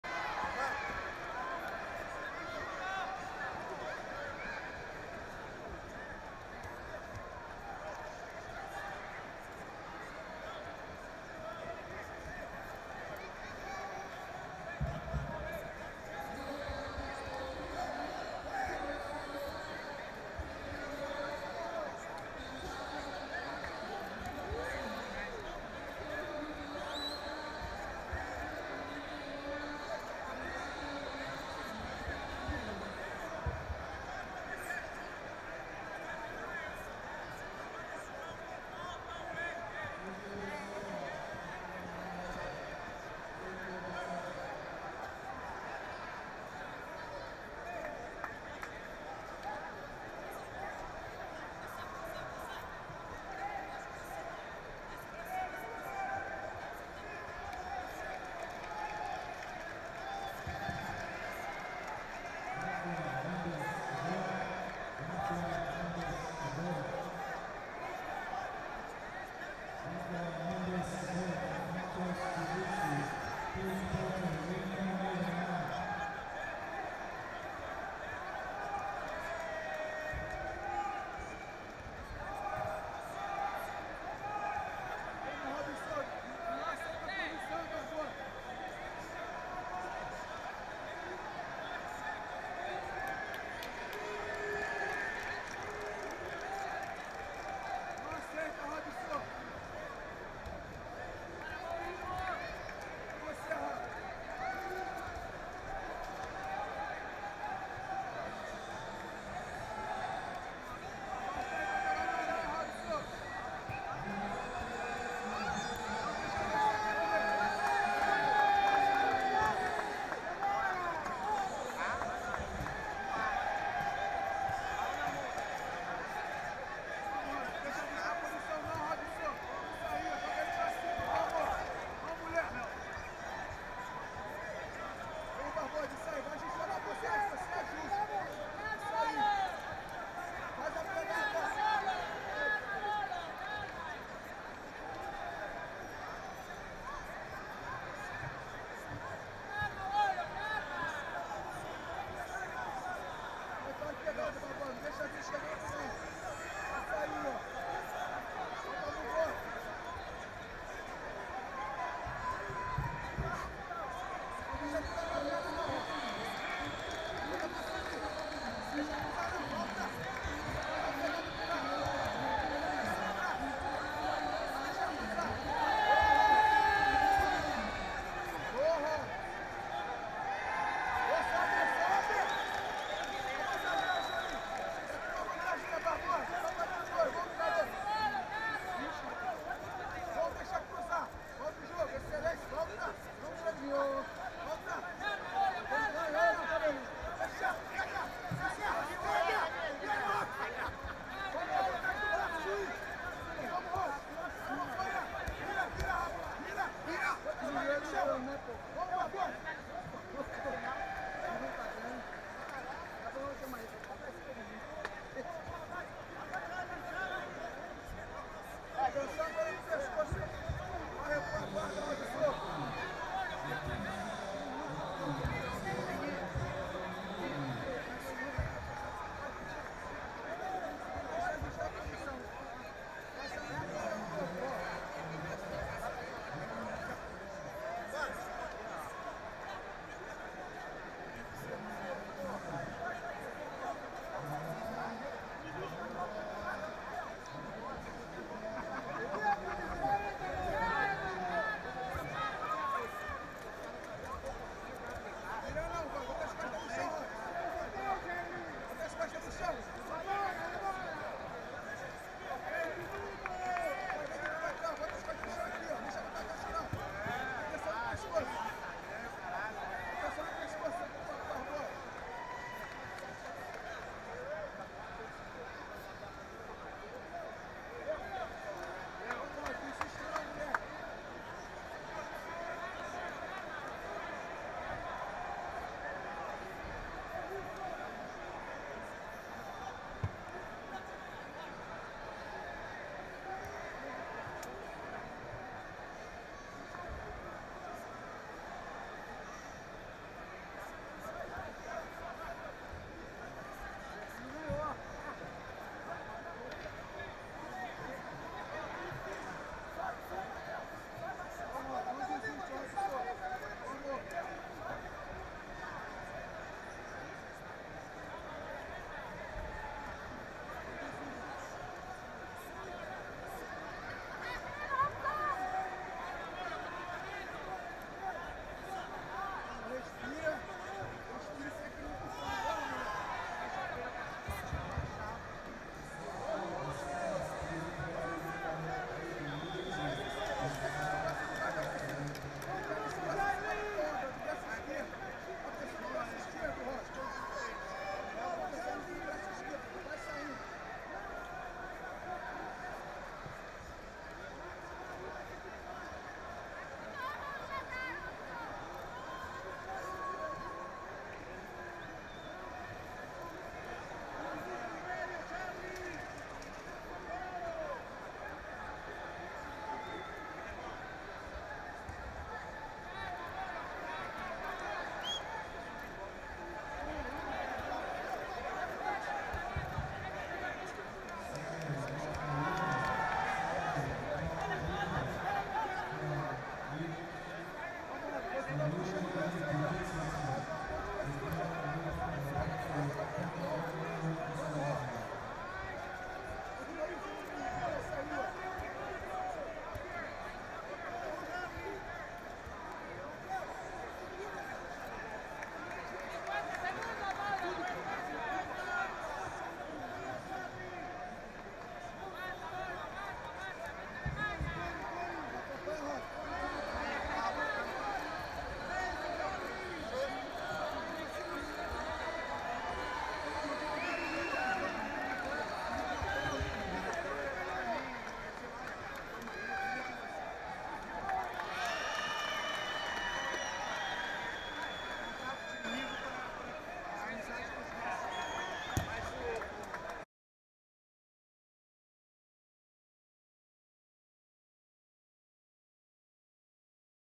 Rubens "Cobrinha" Charles v Robson Barbosa / World Championship 2014
ADULT / BLACK-BELT / MALE / FEATHER - ELIMINATOR